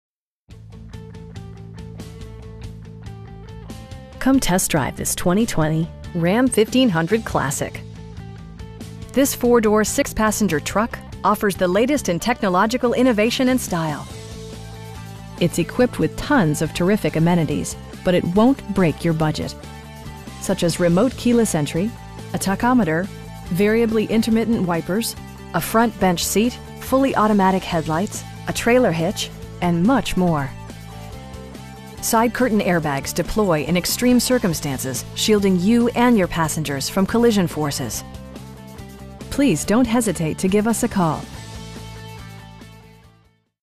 2020 Ram 1500 Classic TRADESMAN QUAD CAB 4X2 6'4 BOX
All American Chrysler Dodge Jeep Ram of Midland

Come test drive this 2020 Ram 1500 Classic. This 4 door, 6 passenger truck offers the latest in technological innovation and style. It's equipped with tons of terrific amenities, but it won't break your budget. Such as remote keyless entry, a tachometer, variably intermittent wipers, a front bench seat, fully automatic headlights, tilt steering wheel, a trailer hitch, and much more. Side curtain airbags deploy in extreme circumstances, shielding you and your passengers from collision forces. Our aim is to provide our customers with the best prices and service at all times. Please don't hesitate to give us a call.